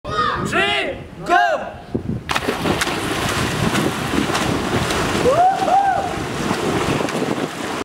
Dive fun in Grand Summer Palace
In 4th sept. 2018 we went to Grand Summer palace . ME (Raju Neupane) along with Praful (TROZEO/ROYAL RECORDS)  , ARUN LAMA TAMANG (KICKERS CREW)
AND KUSHAL SHRESTHA (ROYALRECORDS) Went there for fun
And ended up mAking this video with the help of other peoples who came in pool.